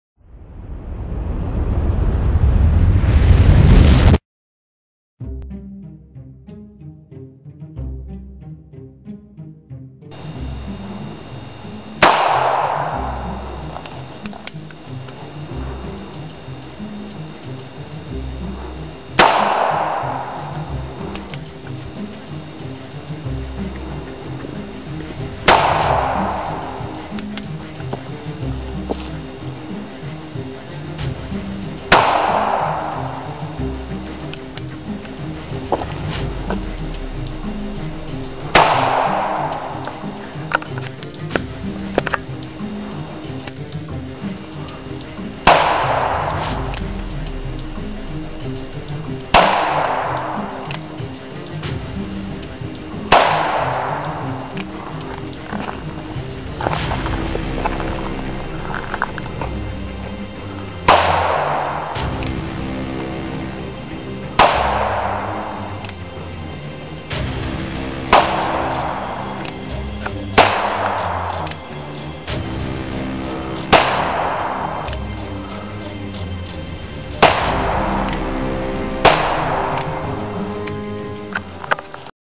.22 Sturm Ruger - Revolver
Shooting the .22 Sturm Ruger. 1 Gun video left to go.